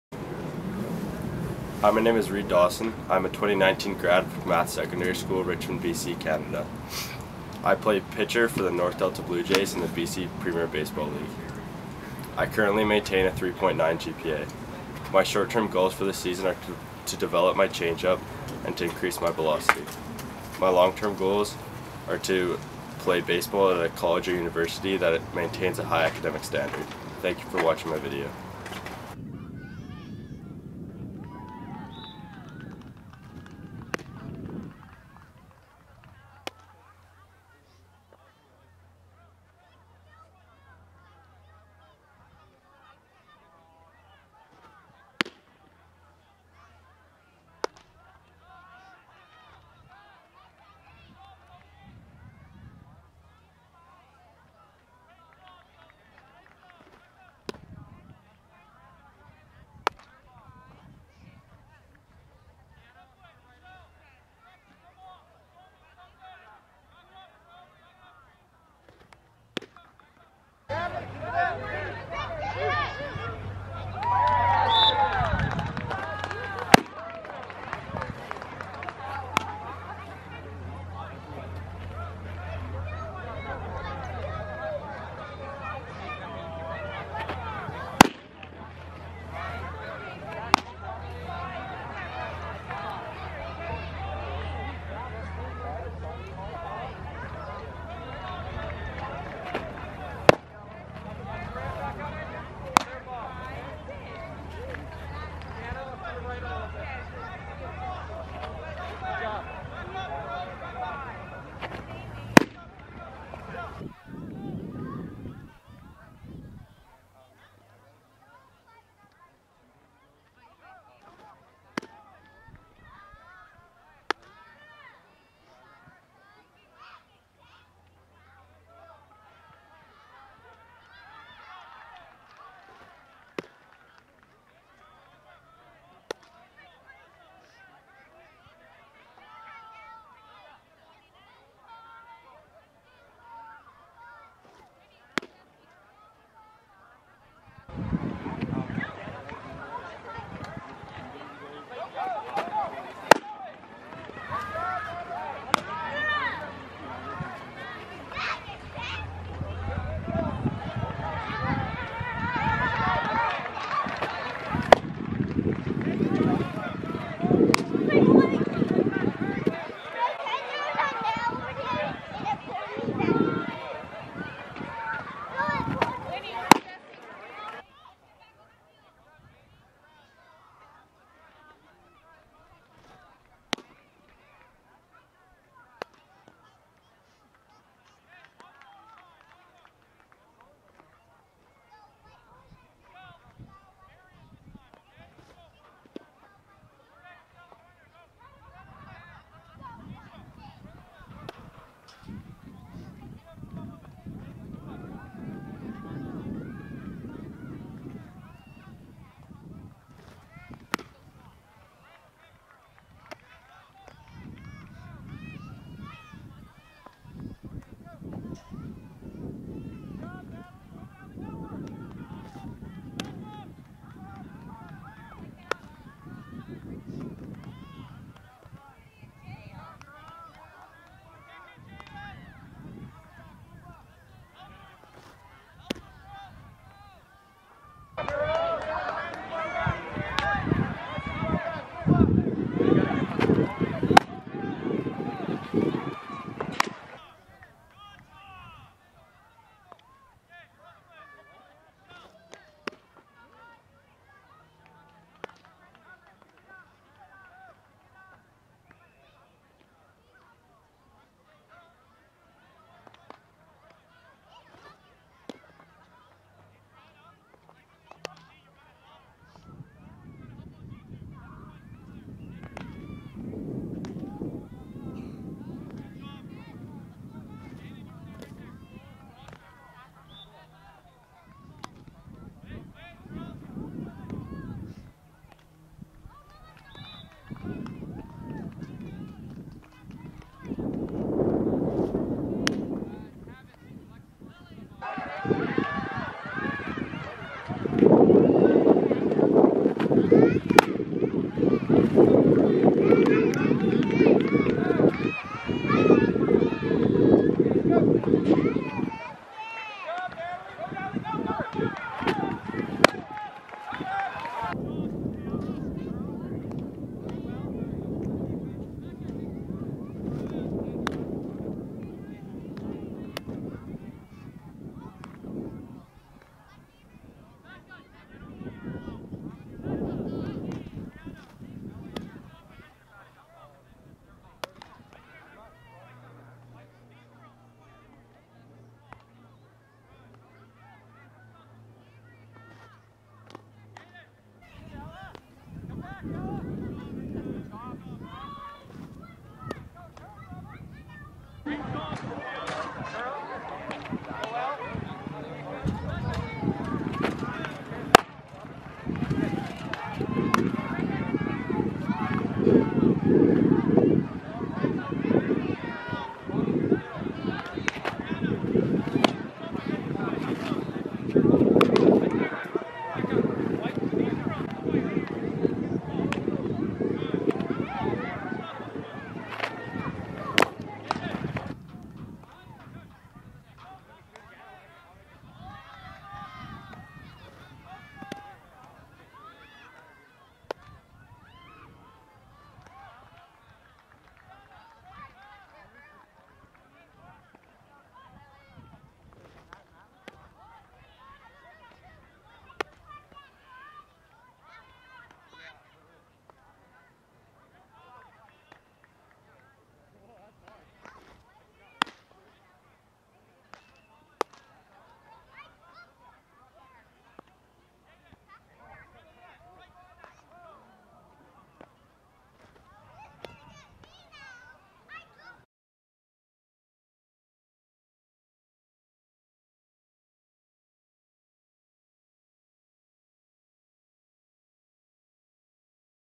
Reid Dawson 2019 RHP
Reid Dawson is a RHP for the North Delta Blue jays of the BC Premier Baseball League. Graduating from McMath Secondary (Richmond, BC), he will be looking to pursue his education and baseball at the college or university level. Currently 6'2" and 160 lbs. (Sept 2017)